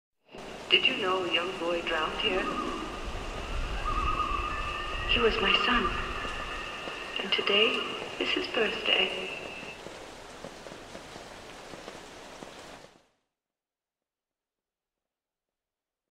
Voorhees Family
A quick piece I threw together practicng making 3D layers from 2D photos in After Effects...